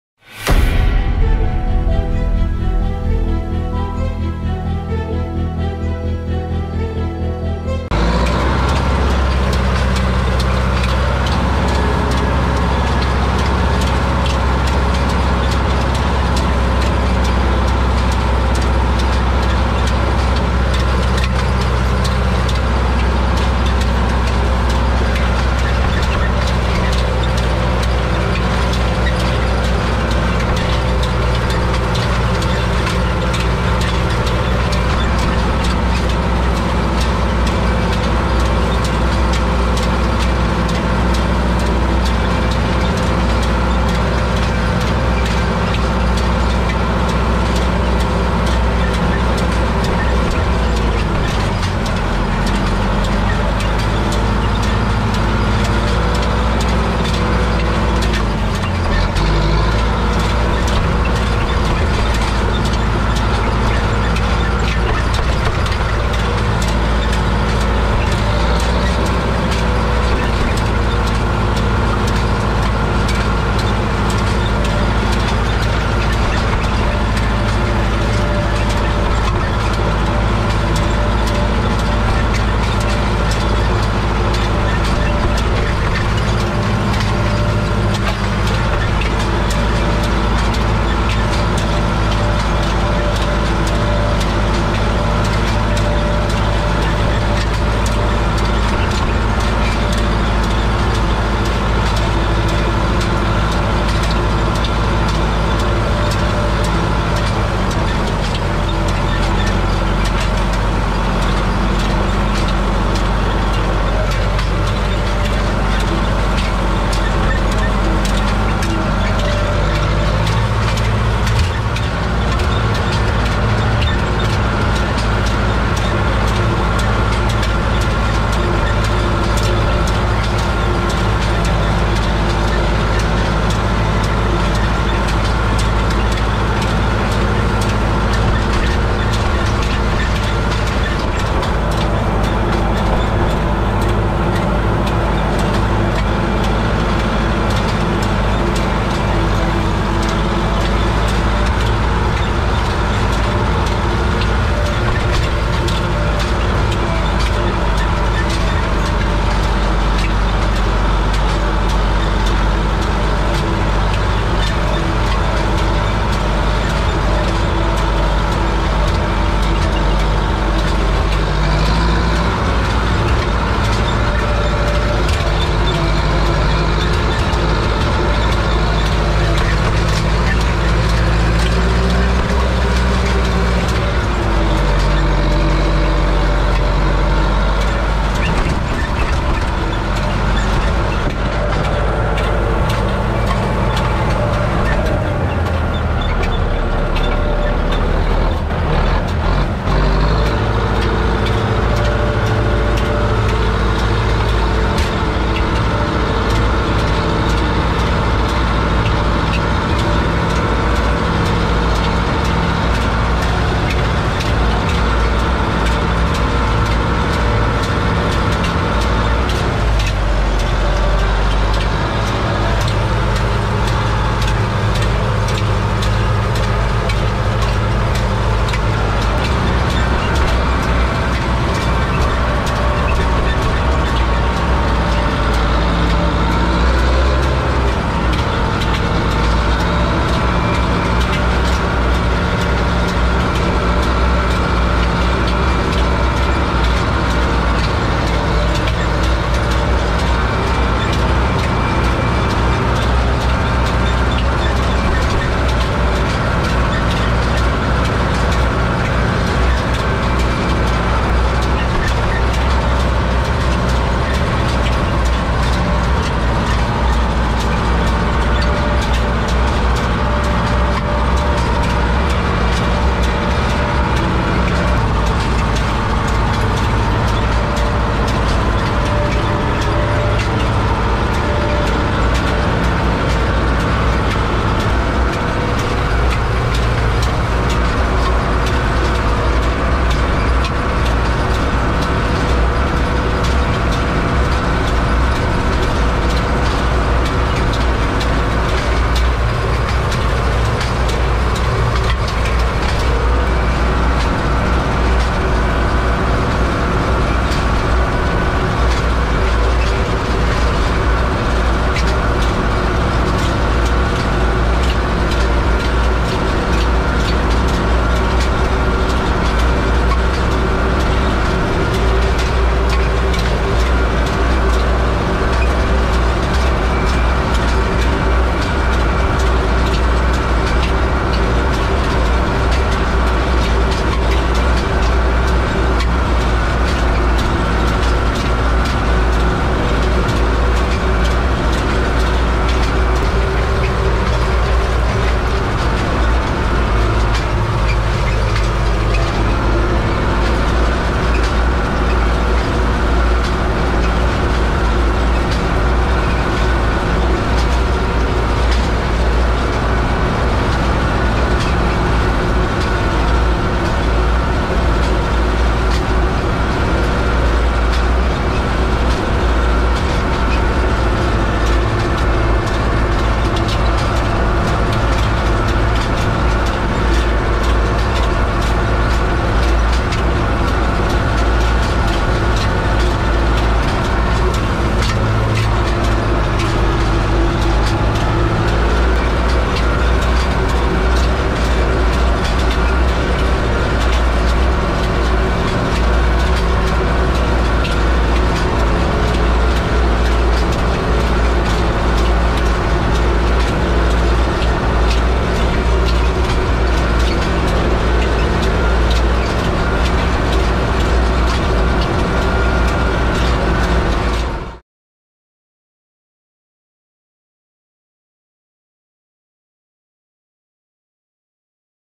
#3 Jak to vidí řidič! I 4K I Hay tedding ! Zetor 6945 + Fella TH 6606 DN ! Gopro Hero 8 Black!
Machines:  Zetor 6945 + Fella TH 6606 DN

Camera: Gopro Hero 8 Black

Programy/Apps: Adobe Premiere Pro CC 2017